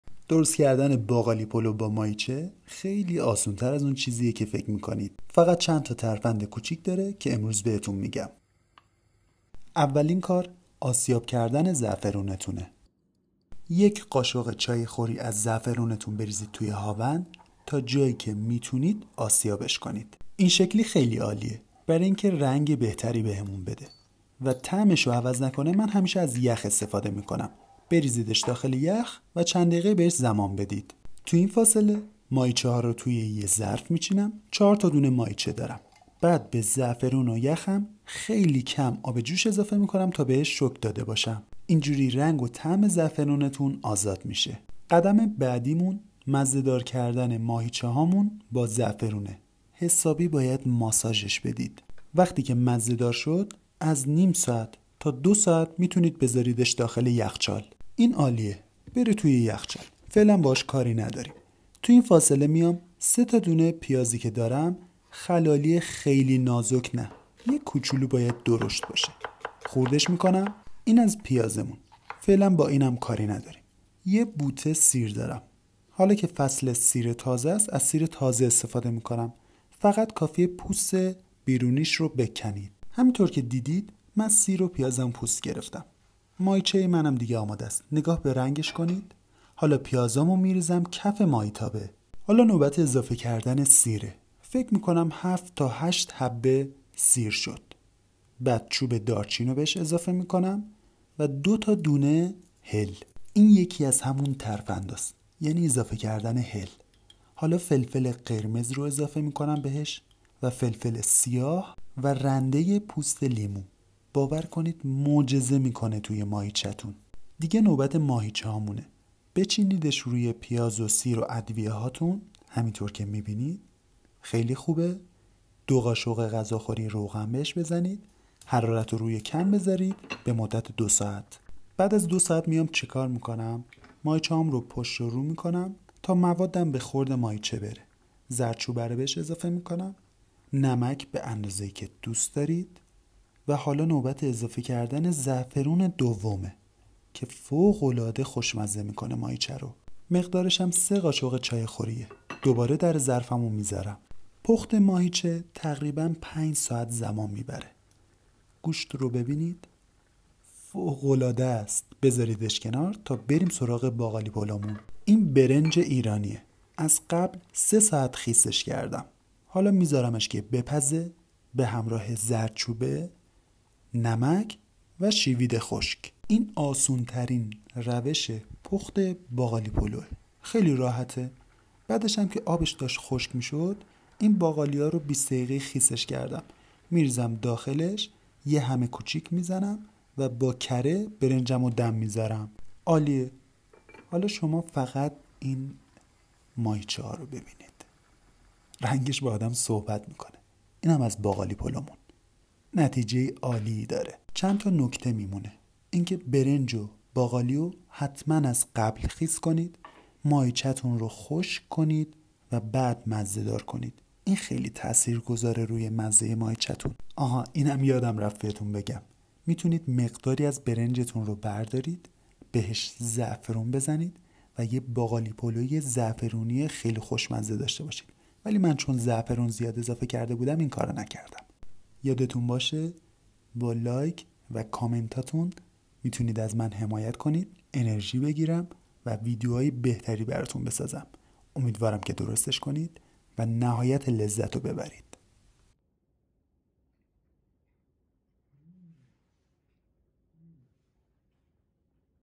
باقالی پلو و با ماهیچه با پیاز کاراملی broad bean rice with lamb shank
آشپزی آموزش ساده ترین و خوشمزه ترین باقالی پلو
۲ قاشق اب جوش بهش اضافه کنید
حالا ۴ تا ماهیچه رو با زعفرونتون مزه دار کنید
نیم ساعت تا ۲ ساعت
بعد ۳ تا پیاز ،۱ بوته سیر ،میخوایم
بریزید کف تابه یا قابلمه چند تا چوب دارچین بریزید روش نوک قاشق فلفل سیاه و قرمز ،حالا ماهیچه ها رو بچینید روش و ۲ ساعت درشو بزارید ،بعد از دو ساعت نمک زرد چوبه و زعفرون رو بهش اضافه کنید و دوباره بزارین بپزه ،حالا برین سراغ برنج ،
۲ ونیم پیمانه برنج رو ۳ ساعت خیس کنید و با یک پیمانه باقالی و ۳ قاشق شوید خشک و نمک و زرد چوبه و کره دم بزارید